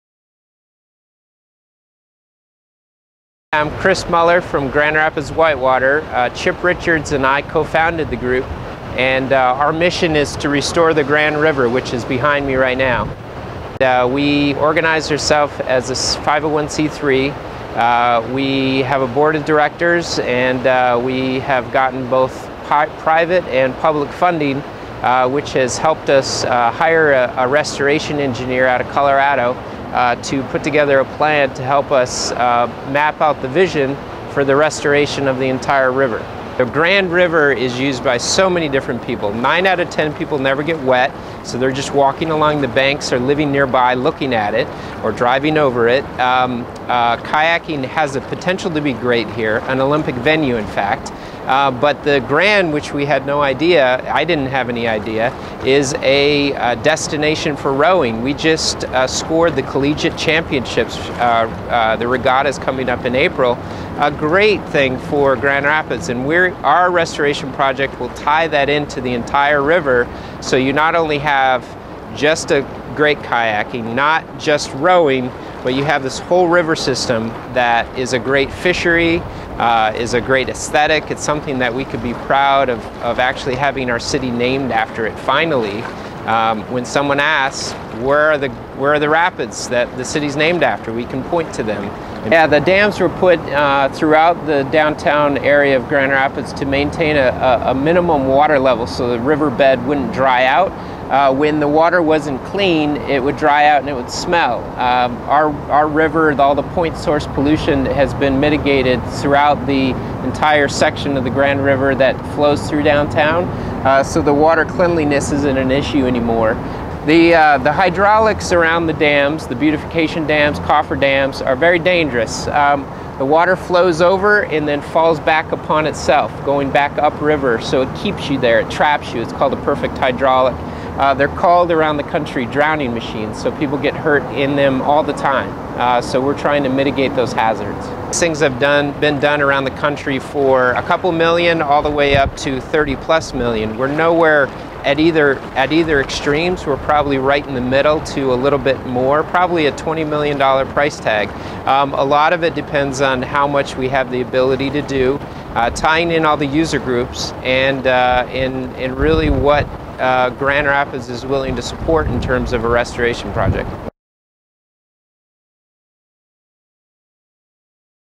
Putting the Rapids back in the Grand
West Michigan business leaders are getting behind a $27 million project that will transform about two miles of the Grand River in downtown Grand Rapids into a churning whitewater course. But the return of the rapids to the Grand River isn't just for paddlers: the restoration will improve fishing, competitive rowing and scenic walkways.